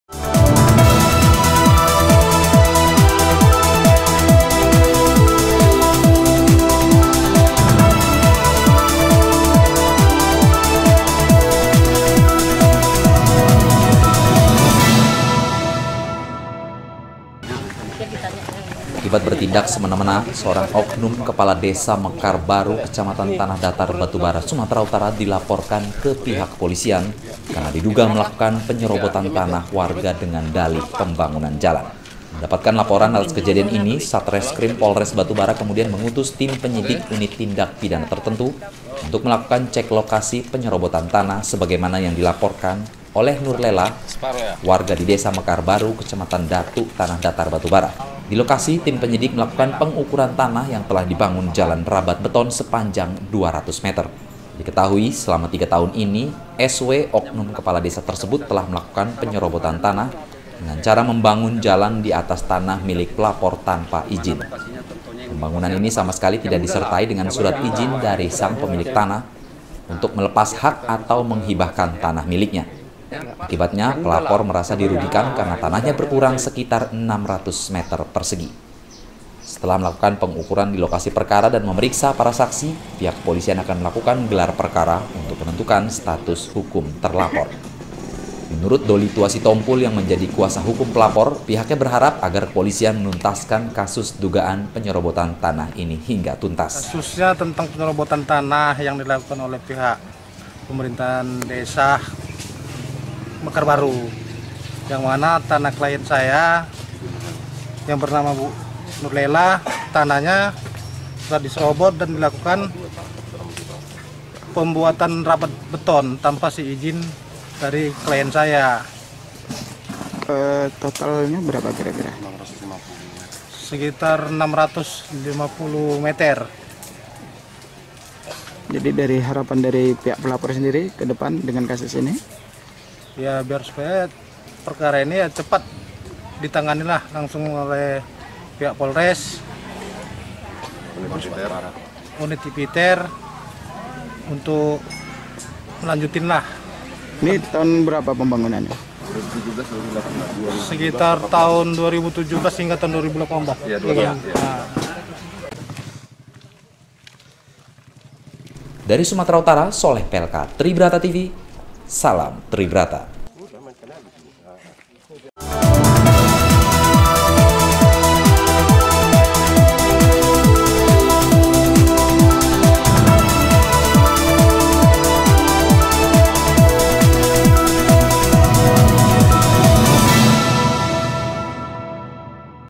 SEROBOT TANAH WARGA OKNUM KADES DIPOLISIKAN
AKIBAT BERTINDAK SEMENA -MENA SEORANG OKNUM KEPALA DESA MEKAR BARU KECAMATAN TANAH DATAR BATU BARA SUMATERA UTARA DILAPORKAN KE POLISI KARENA DIDUGA MELAKUKAN PENYEROBOTAN TANAH WARGA DENGAN DALIH PEMBANGUNAN JALAN.